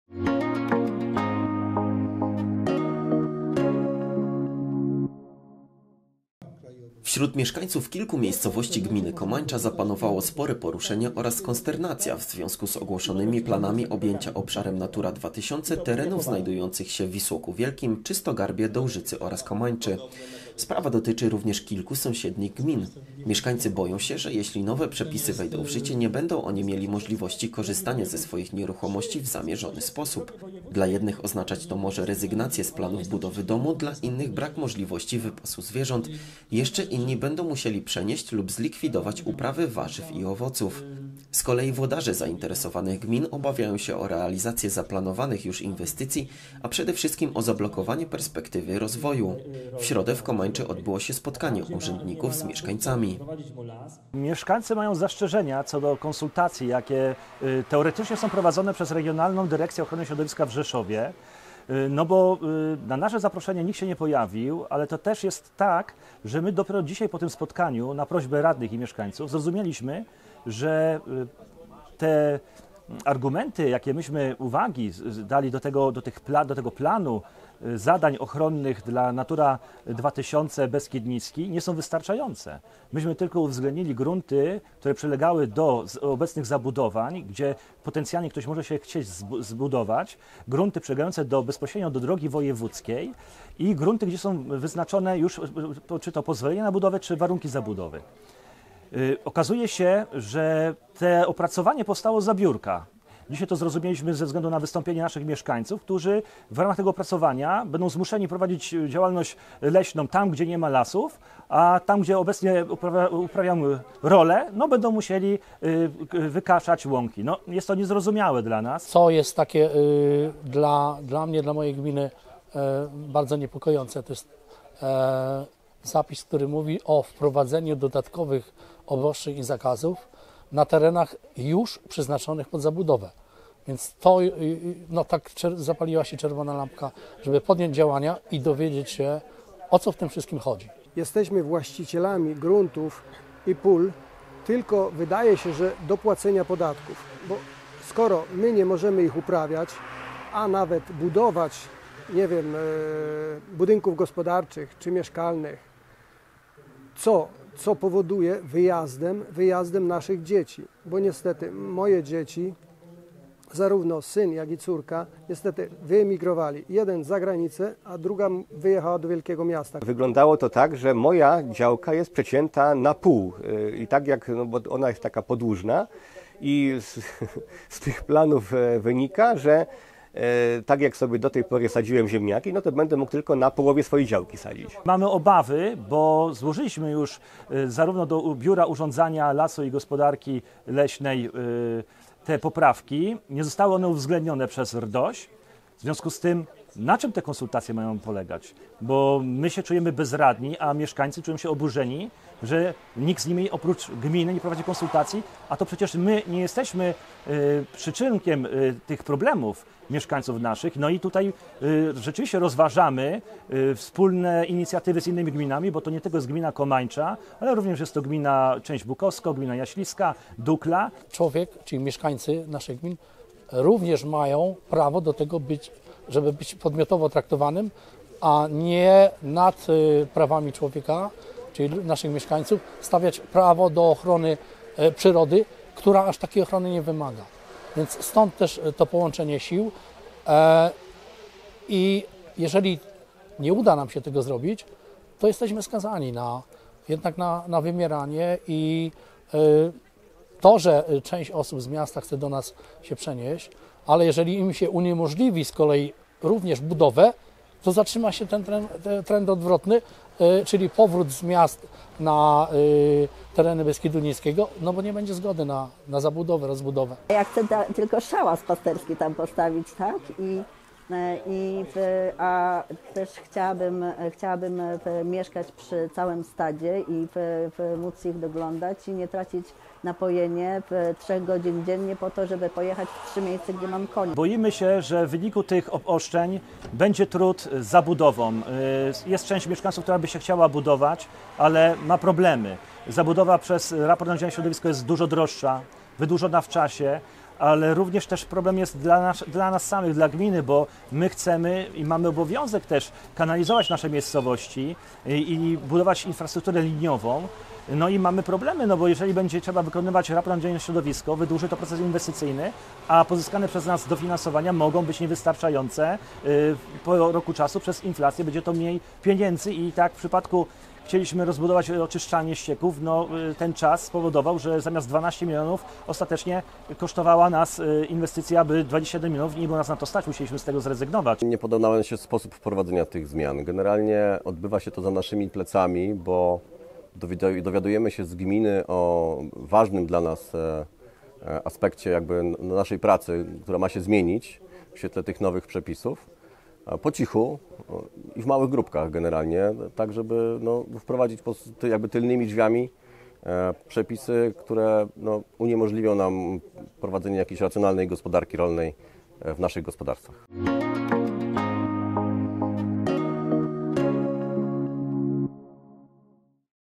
KOMAŃCZA. Protest mieszkańców. "Bez rozwoju, znikniemy!"
GMINA KOMAŃCZA / PODKARPACIE. Wśród mieszkańców kilku miejscowości gminy Komańcza zapanowało spore poruszenie oraz konsternacja w związku z ogłoszonymi planami objęcia obszarem Natura 2000 terenów znajdujących się w Wisłoku Wielkim, Czystogarbie, Dołżycy oraz Komańczy. Sprawa dotyczy również kilku sąsiednich gmin.